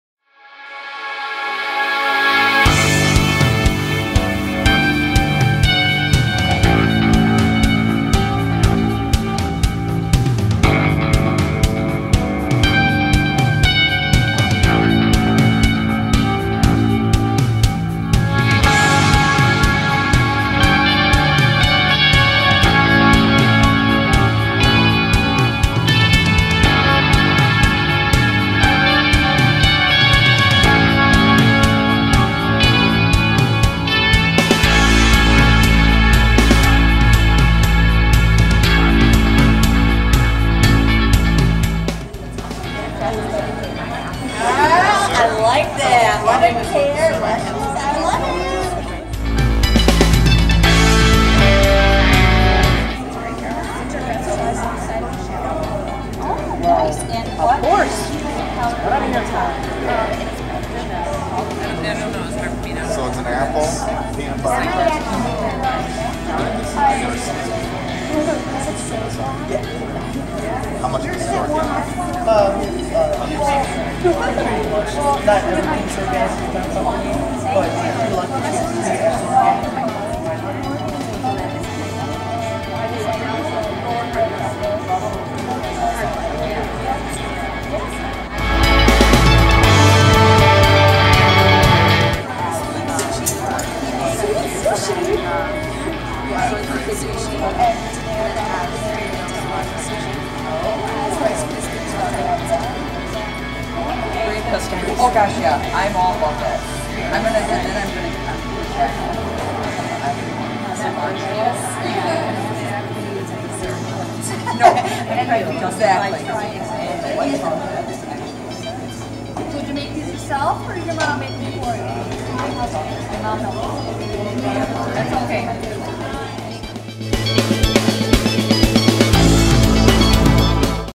Food & Snack Convention (Qtr. 2, 2015-16)
The Food & Snack Convention for the 8th Gr. Business Class at Bessie Allen Middle School at North Fond du Lac (from quarter 2 of the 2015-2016 year)